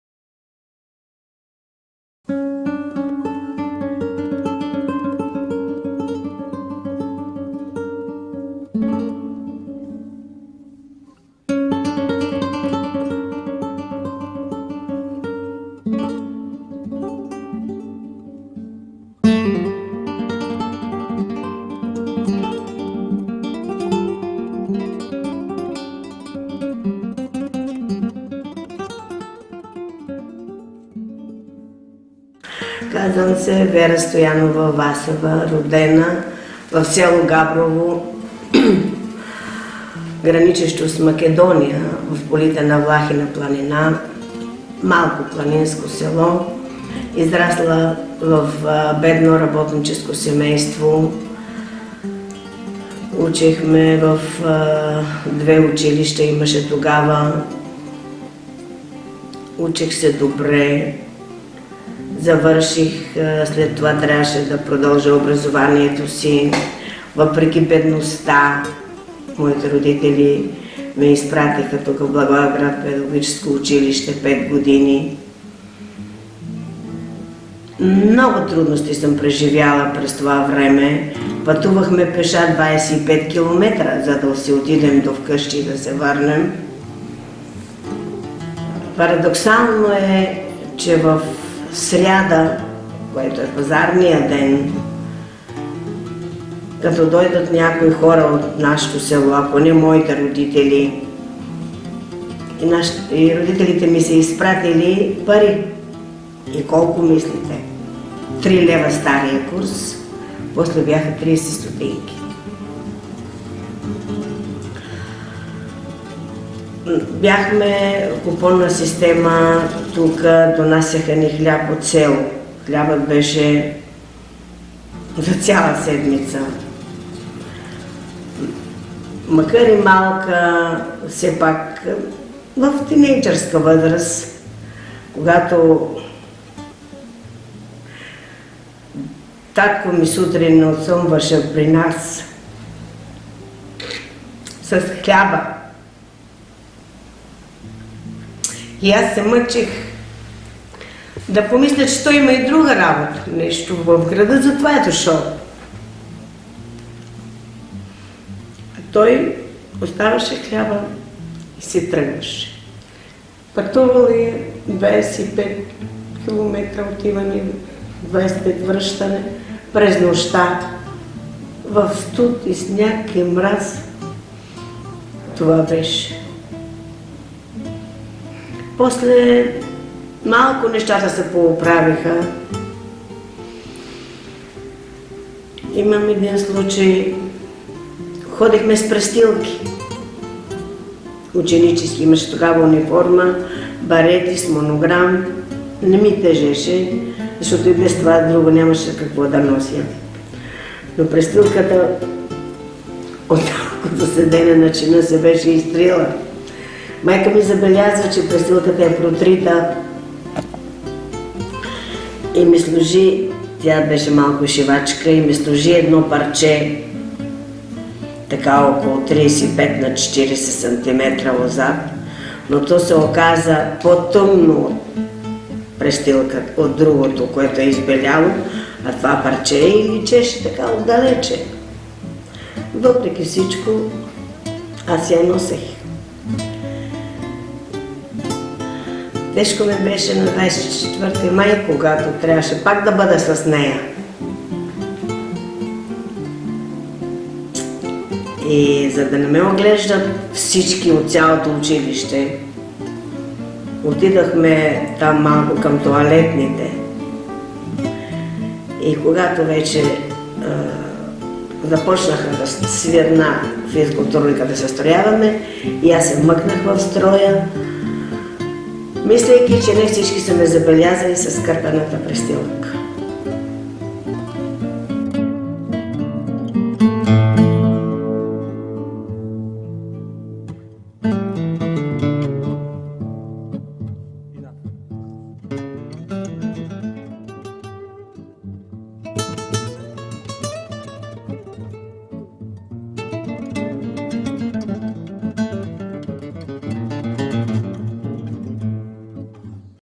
Vera Stoyanova: The Story of the Mended Uniform
Vera Stoyanova is a woman who fought hard to get out of the poverty cirle.Born in the village of Gabrovo in a poor workers' family, Vera is the first one to finish higher education.
Вера Стоянова е жена, която се бори ожесточено да се измъкне от кръга на бедността. Родена в село Габрово, Вера е първата в своето семейство, която завършва висше образование.